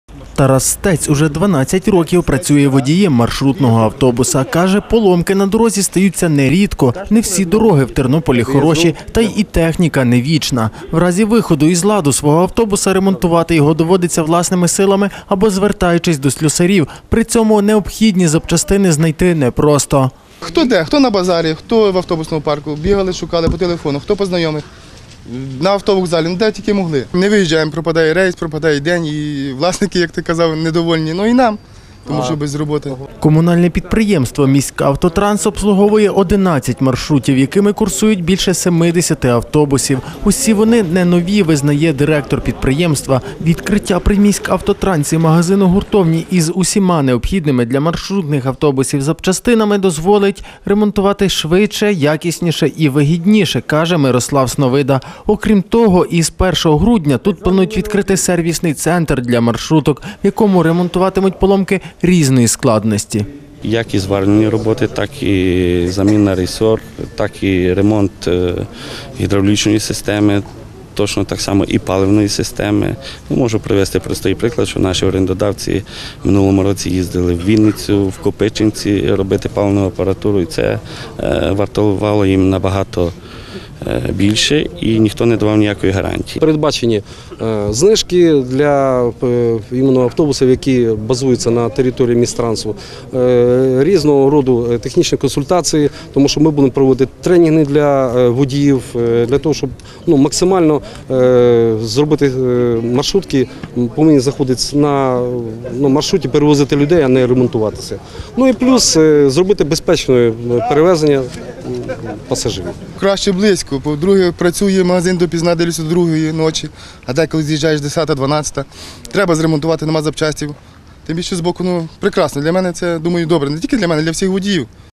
Магазин-гуртовню сертифікованих запчастин для маршрутних автобусів відкрили у Тернополі
Дешевше, швидше і якісніше ремонтуватимуть маршрутки у Тернополі. При комунальному підприємстві "Міськавтотранс", яке здійснює маршрутні перевезення в місті, сьогодні відкрили магазин-гуртовню сертифікованих запчастин для маршрутних автобусів, які раніше шукали і замовляли хто де. Водії маршруток кажуть, вчасний і надійний ремонт дозволить уникати перебоїв у графіку та зробить поїздки безпечнішими.